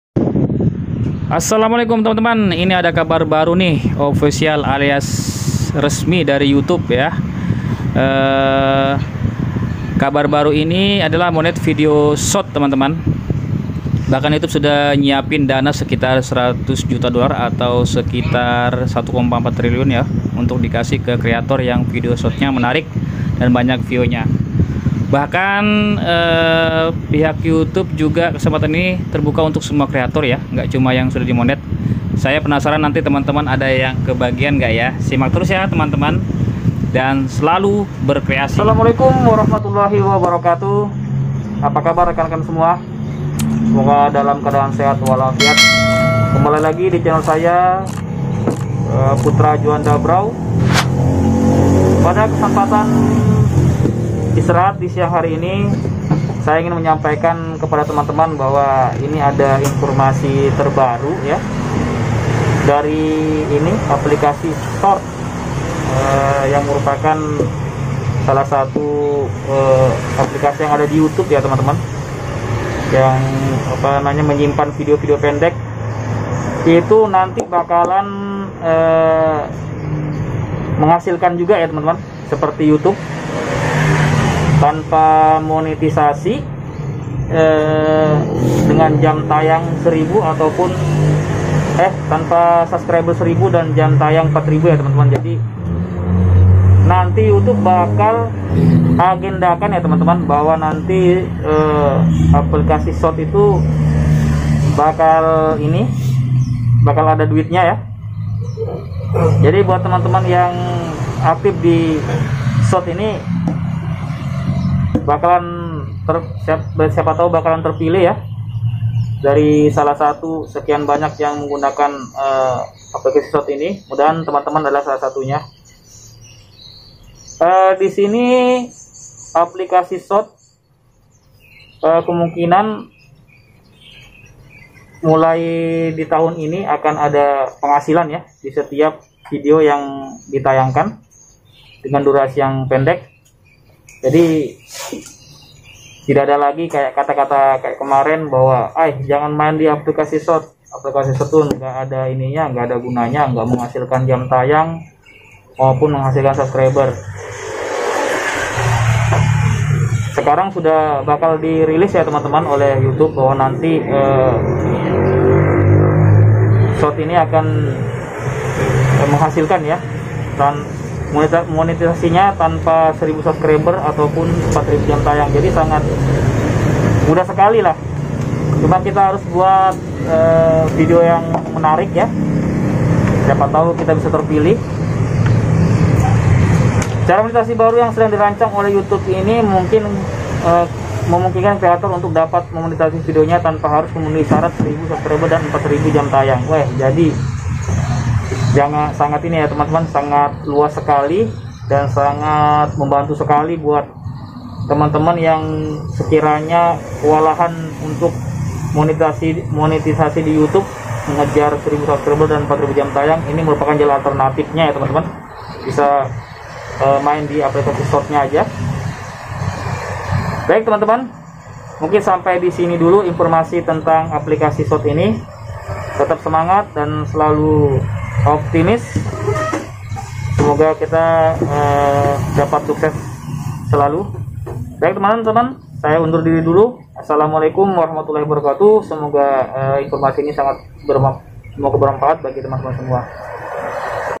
Info Terbaru Tanpa 1000 Sub Dan 4000 jam tayang Di Monetisasi
assalamualaikum sahabat ku.
ada info terbaru yg sedang di rancang oleh yutube untuk dapat di monitisasi vidio nya tanpa harus memenuhi persyaratan 1000 subcreber dan 4000 jam tayang.

gabung temen ngopi Den JC:http:/ ngopi DenJC